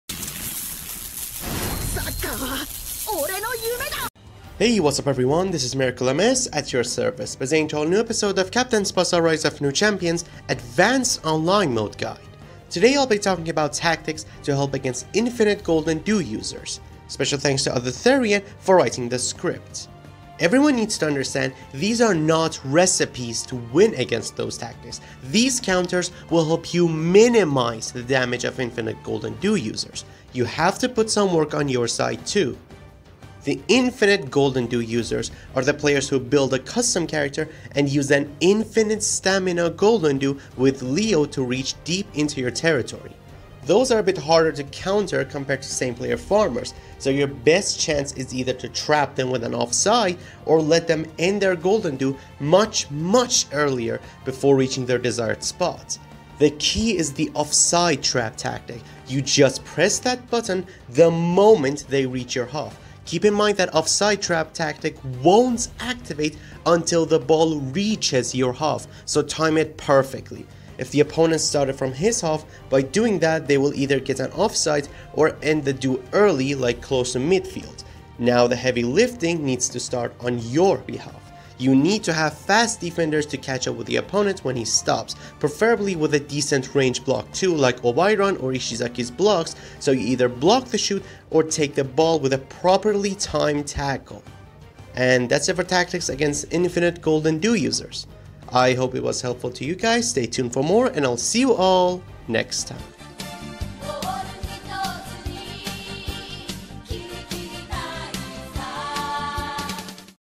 Online Advanced Guide: Stopping Infinite Golden Duo
Part 2 of Anti Cheap Playstyles! There will be 2 more parts for this: No Dribble Long Shooters and Birdcagers!